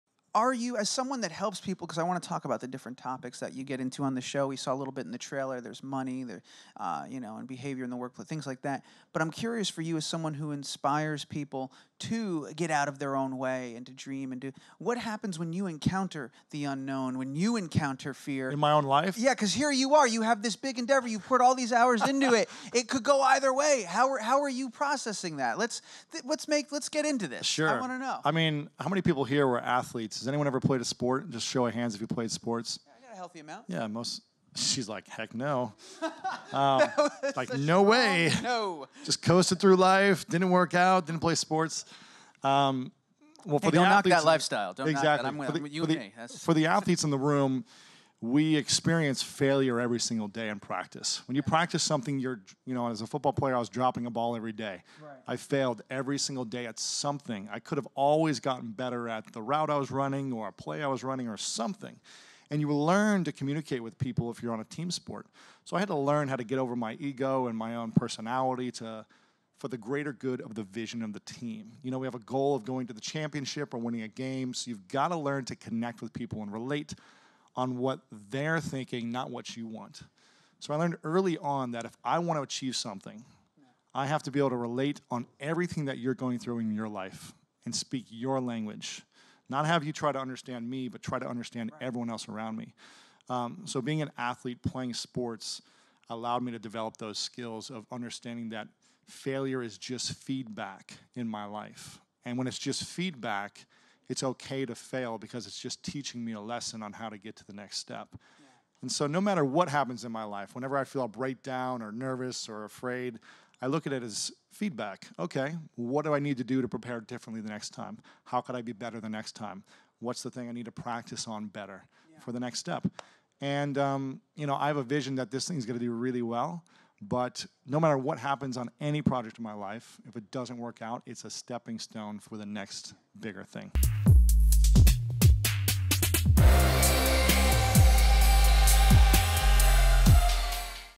How Lewis Howes Deals With The Unknown
Lewis Howes discusses how he encounters fear and his approach to handling the unknown.